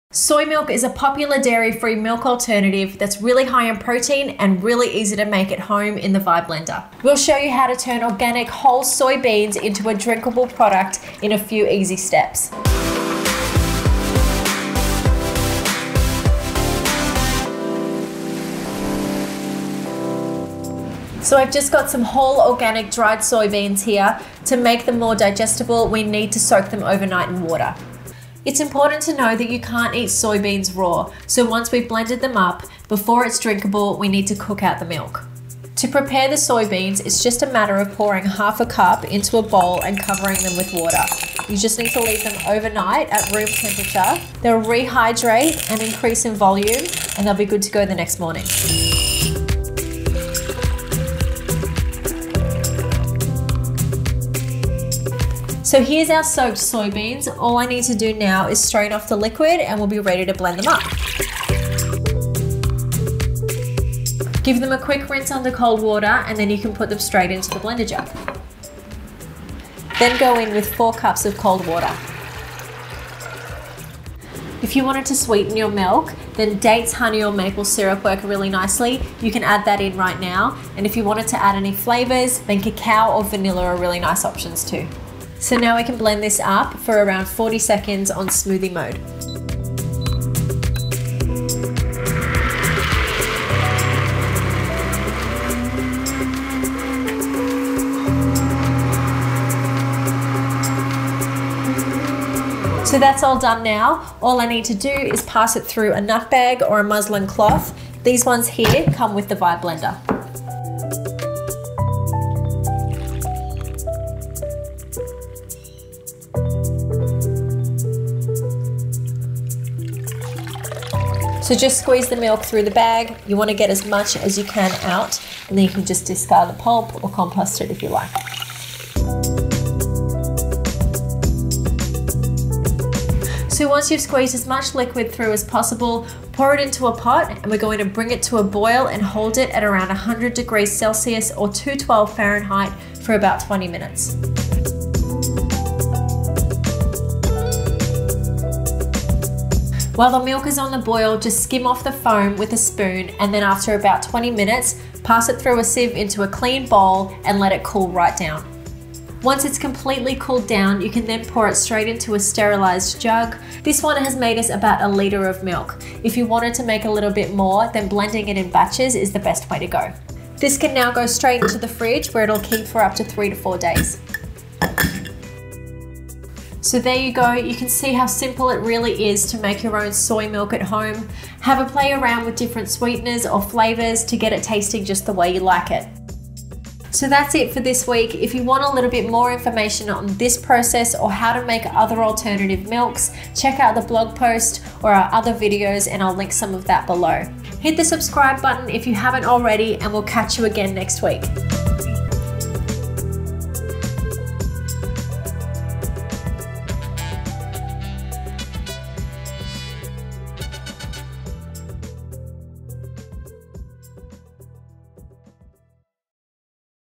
Step by step HOMEMADE SOY MILK with a blender
Soy milk is a popular lactose and dairy free alternative to cow’s milk. It’s plant-based, high in protein, compares nutritionally to cow’s milk and can be used in place of dairy milk in beverages, cooking and baking.
by making soy milk at home with organic soybeans, you’re able to control exactly what goes into your milk. With the Vibe Blender system, making soy milk is an easy process, using just two ingredients – soybeans and water. Buying soy beans in bulk is also very cost effective.